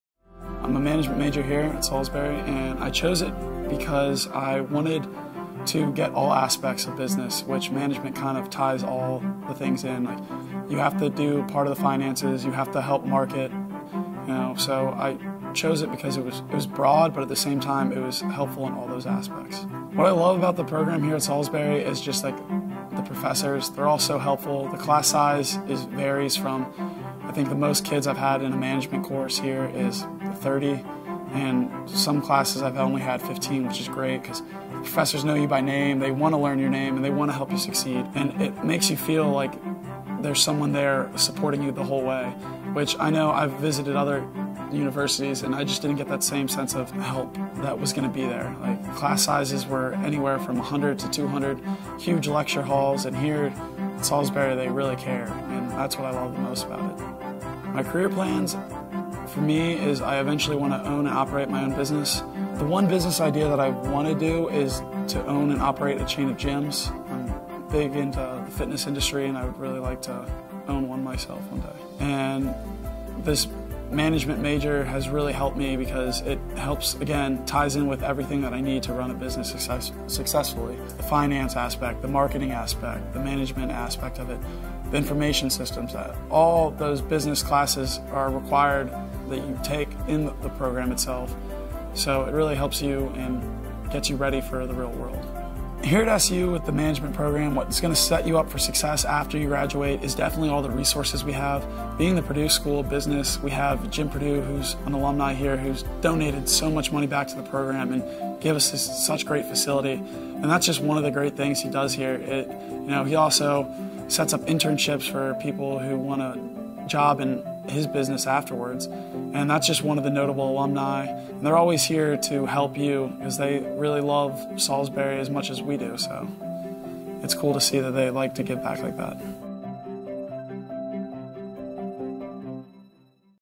Meet Nick – Management Major at Salisbury University
Find out how SU’s management major is preparing Nick to own his own business someday.